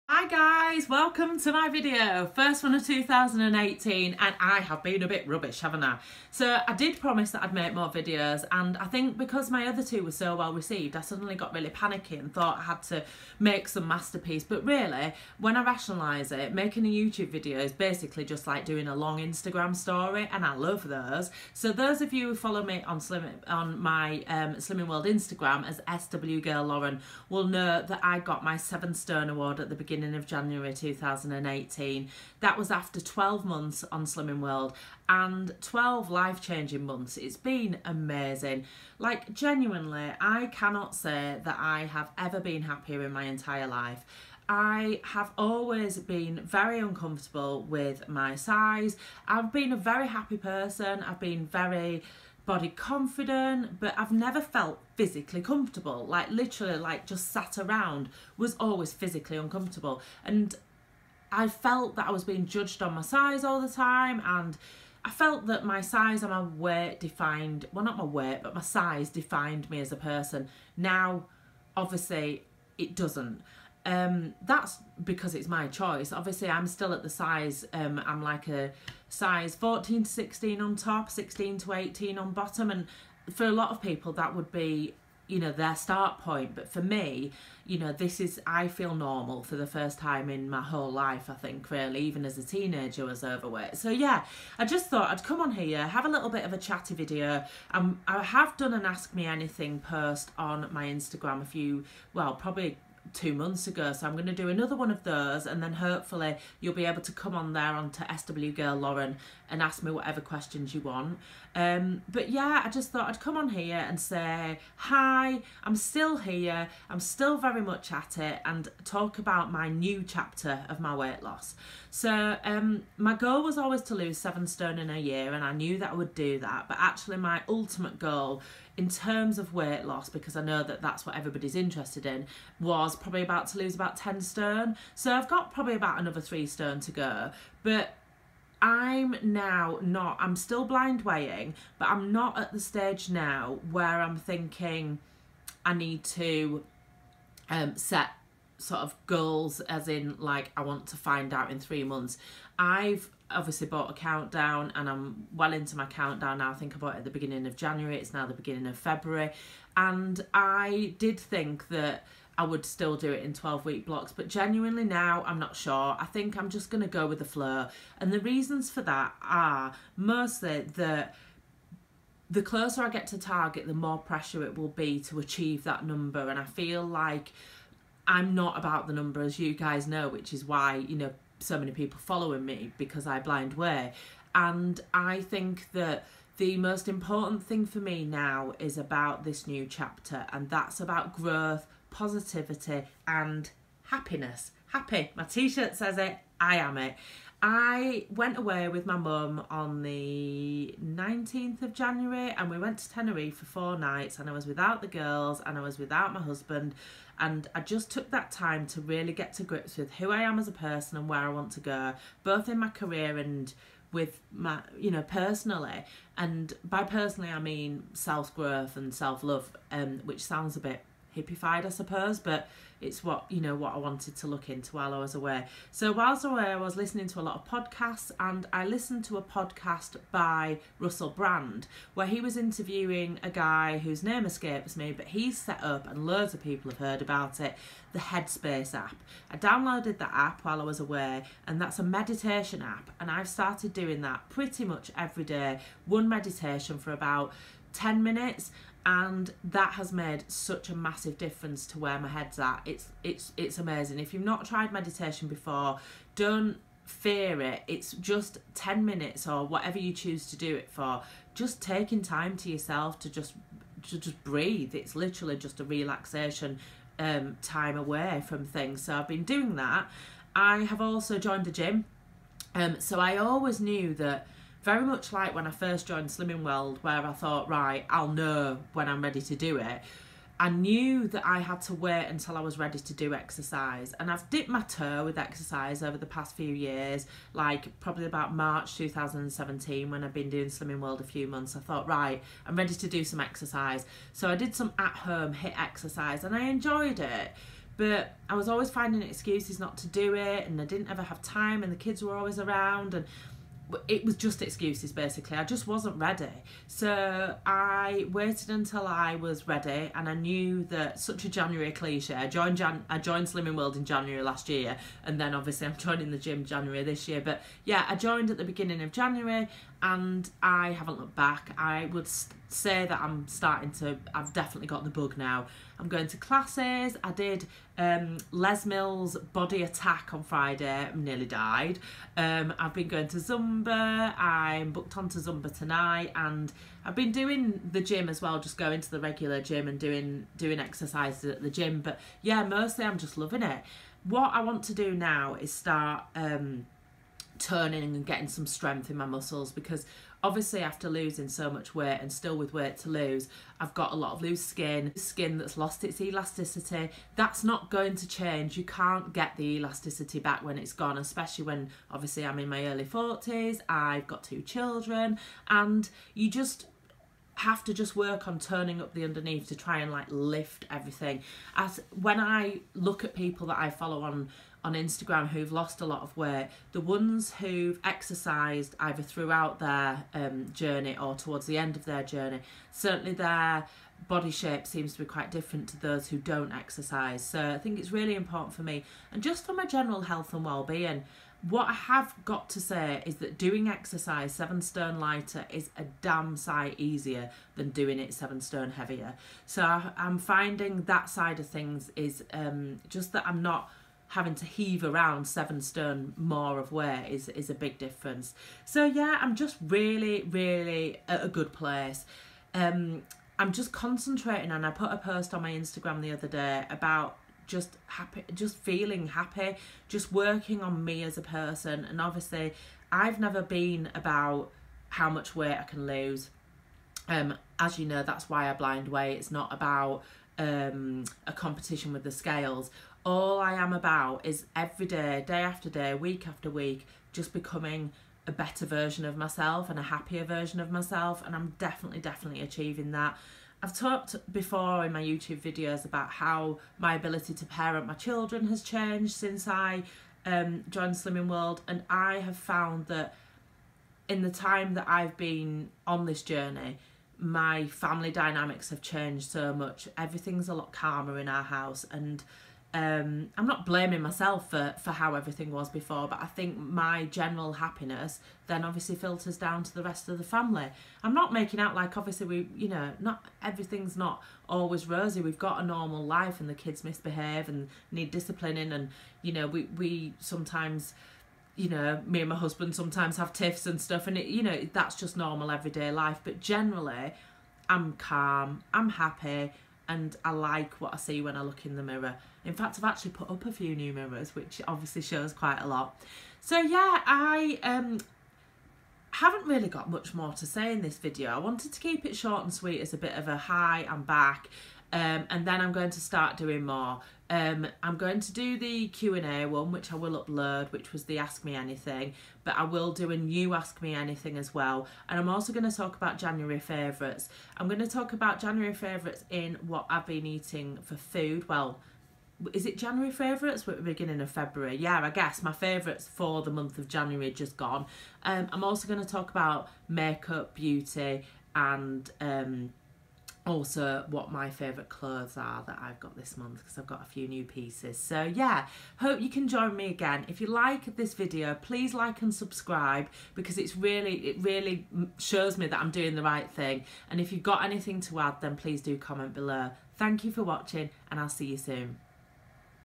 Slimming World 1 year on... I lost 7 Stone!
Thanks for all your support.  So, I did it! 7 stone 1.5 lbs lost in 12 months.  Just a chatty little video talking about where I'm at and what are my weight-loss plans moving forward.